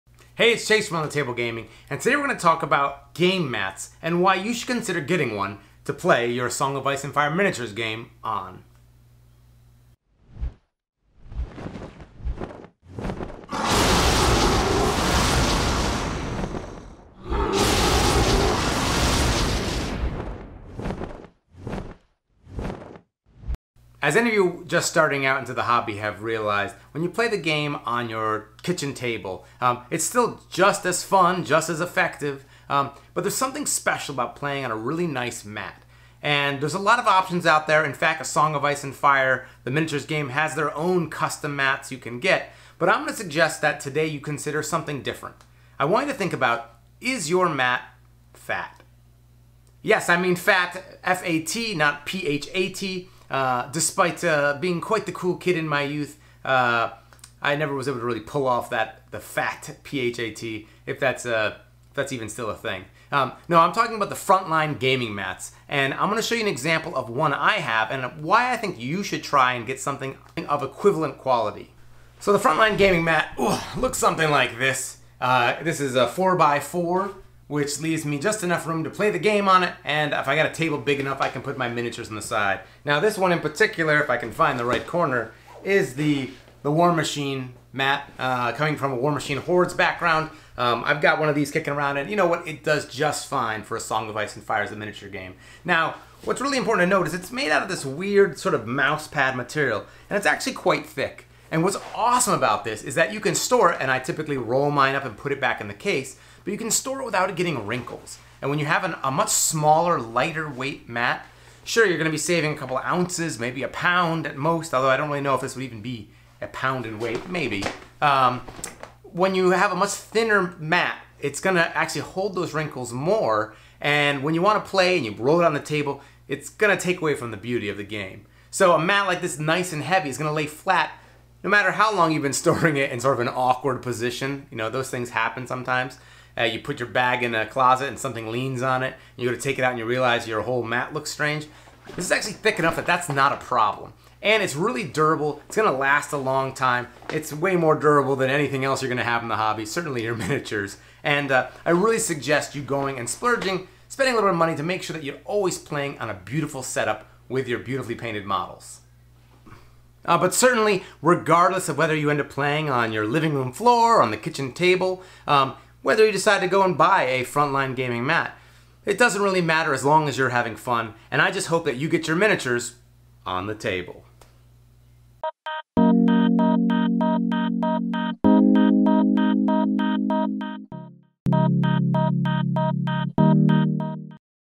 FLG Gaming Mats for ASOIAF?
Chase from On the Tabling Gaming looks at Front Line-Gaming's "F.A.T" mats. (Please note, FLG is moving away from the "F.A.T" title to "FLG Mats"  - but the quality remains!)